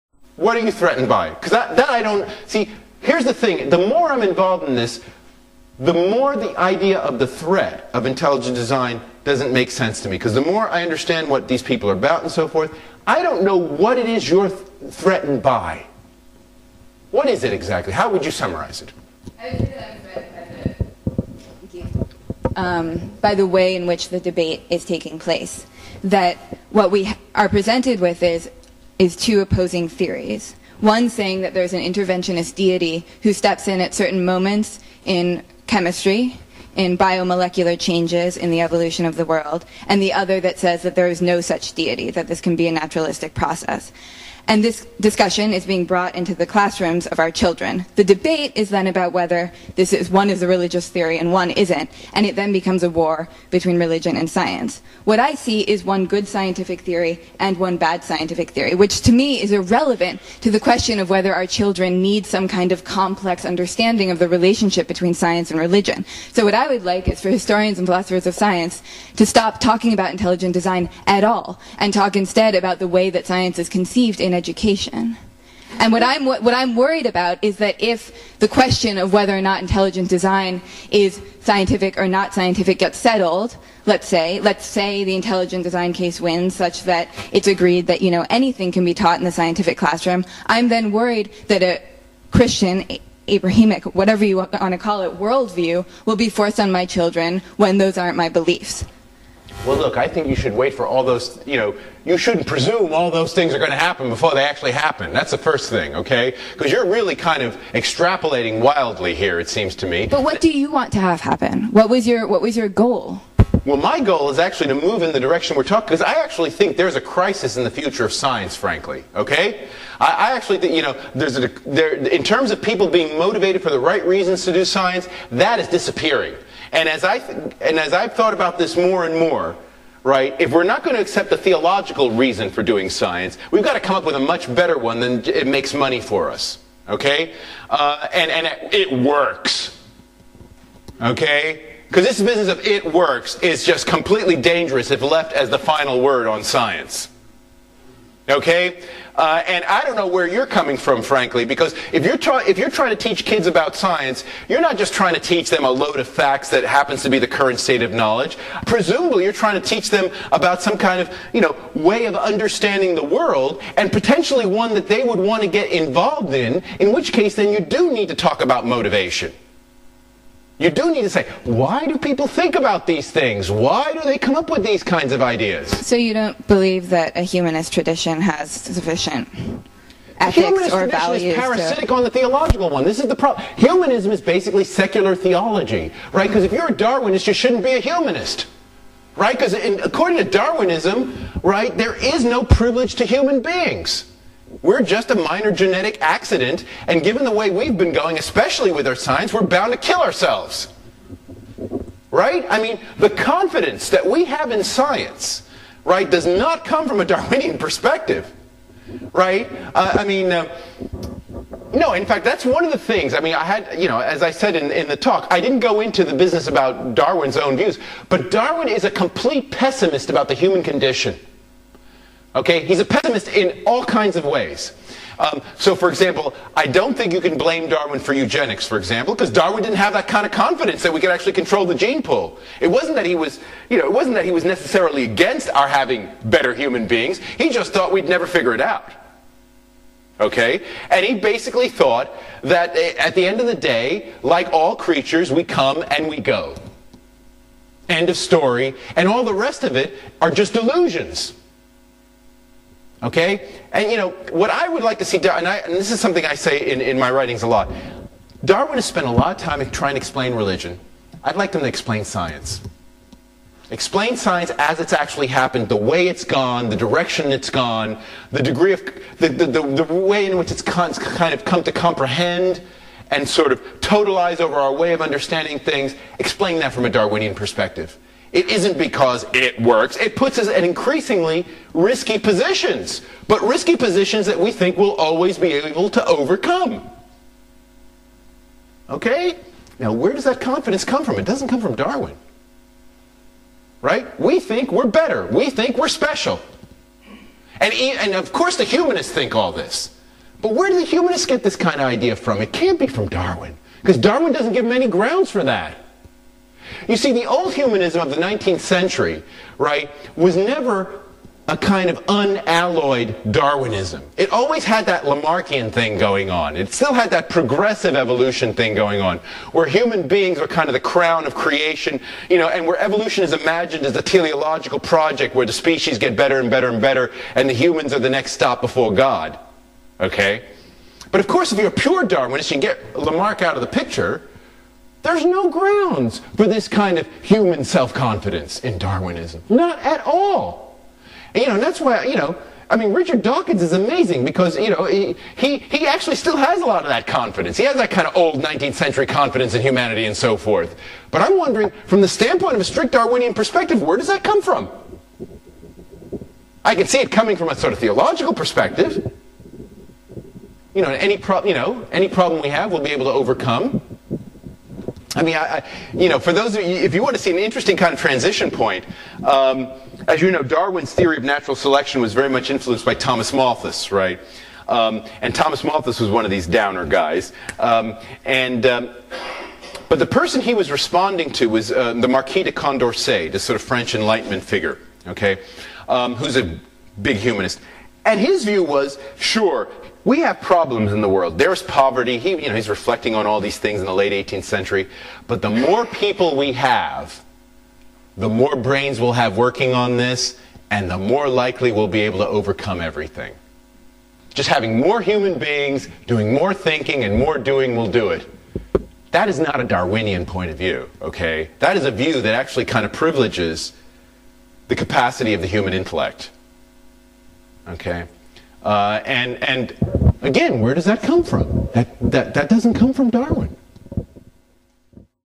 Why are you threatened by Intelligent Design? by Professor Steve Fuller of Warwick University
Professor Steve Fuller of Warwick University
From the lecture: 'Dissent Over Descent: Intelligent Design's Challenge to Darwinism'
Filmed July 11 2009 at Cambridge University